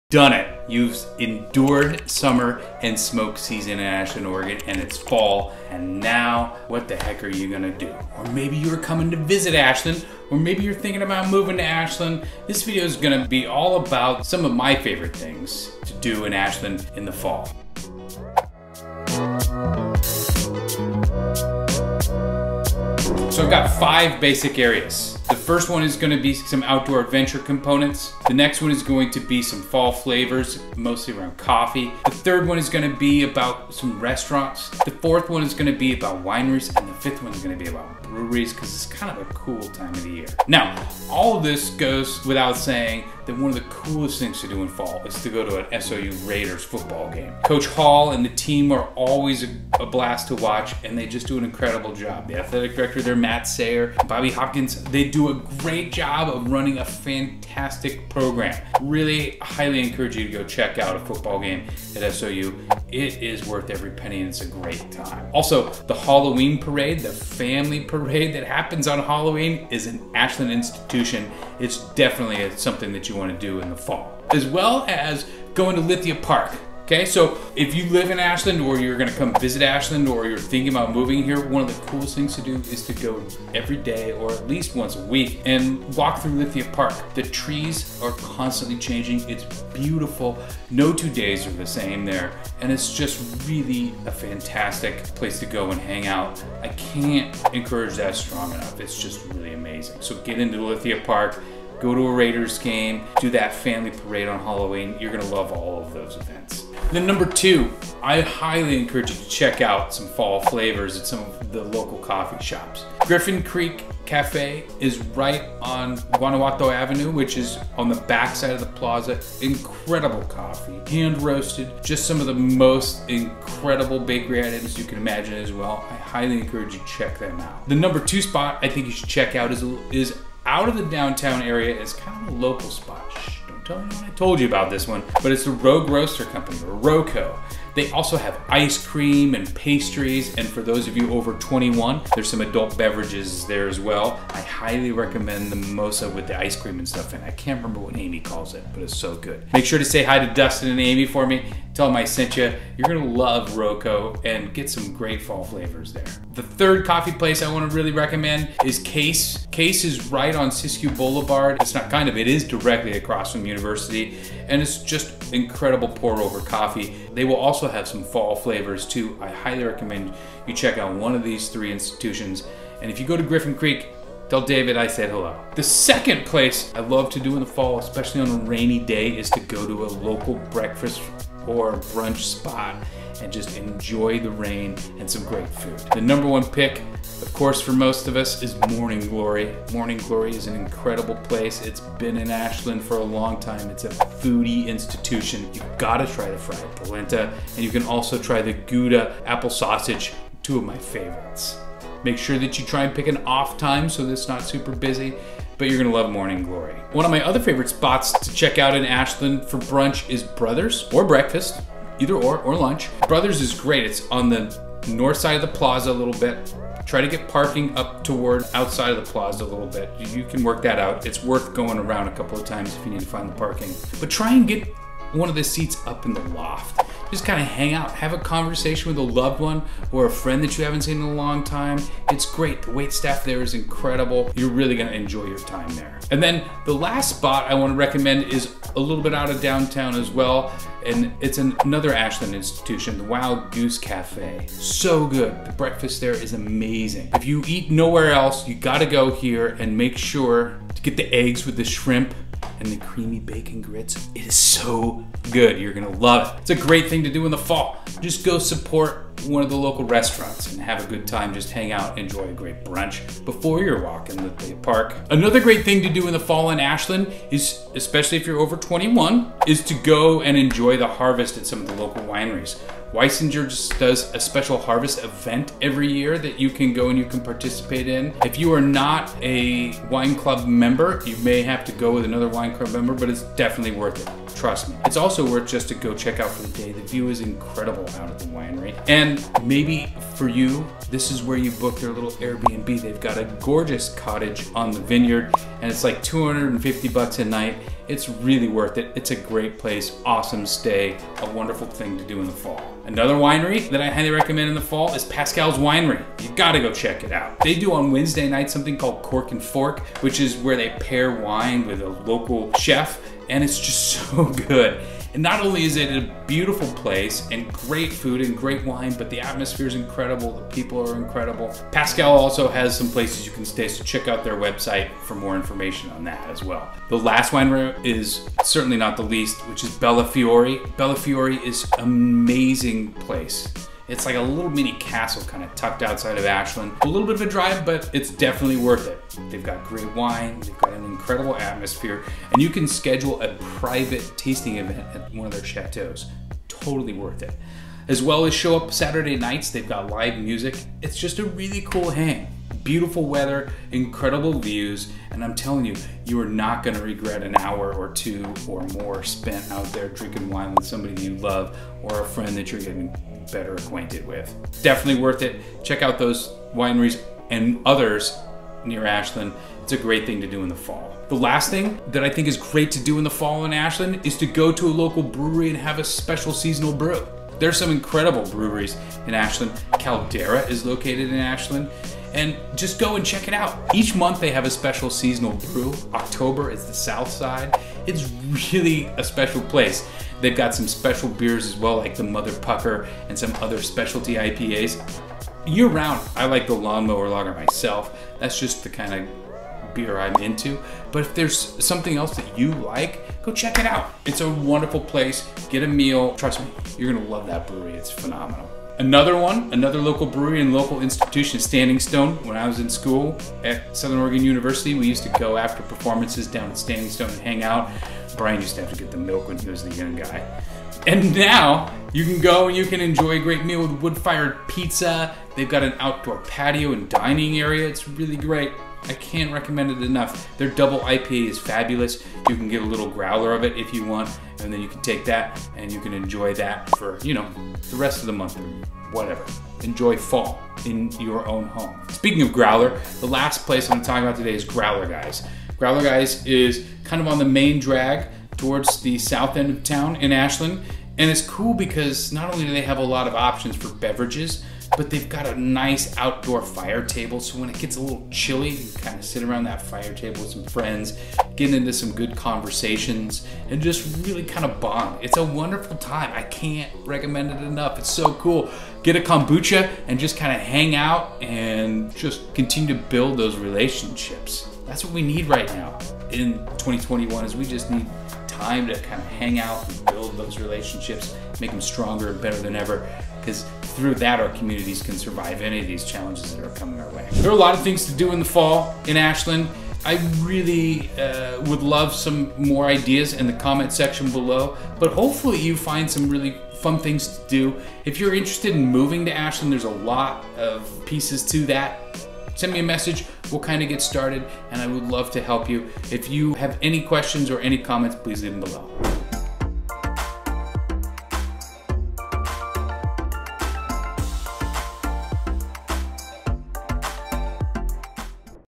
Things to do in Ashland Oregon - THE FALL!
In this video 📽, I highlight some fun things to do in Ashland Oregon in the fall! Enjoy, and add to your own list. Is there something fun that I missed?

To get the MOST UP TO DATE ⏰ (cause we all know Zillow is just not up to date) list ✍️ of homes currently on the market today, go to my